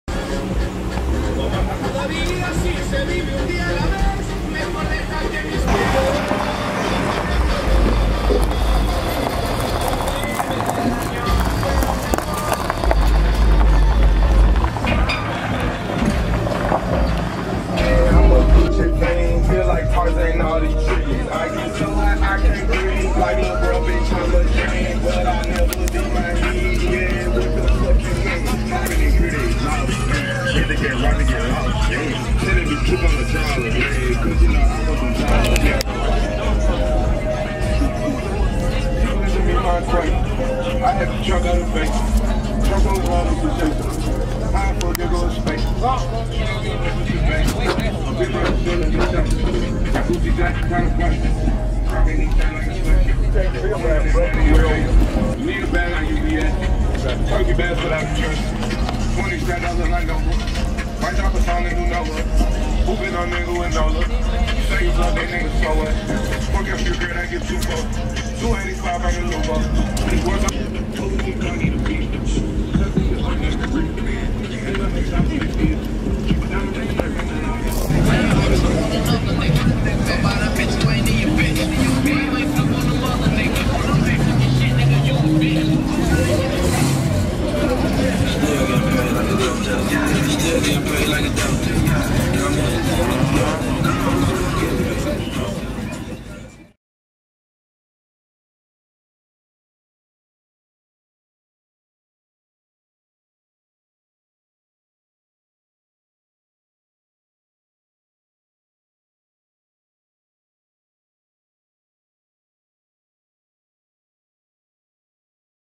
WhipAddict: WhipAddict: Blue on Blue Chevy Silverado Short Bed on DUB Ballers 28s, Custom Interior
KingOfTheSouthCarShow: Chevrolet Silverado Short Bed C10, Navy Blue body with light blue highlights in the grille, under the bumper, on the emblems and bumper rivets. Inside is the same color scheme with ostrich and leather with custom console. In the bed are two nitrous bottles and the alternating blue lines. Under the hood is a clean small block with a lot of chrome on it! All sittin on custom painted 28" DUB Ballers!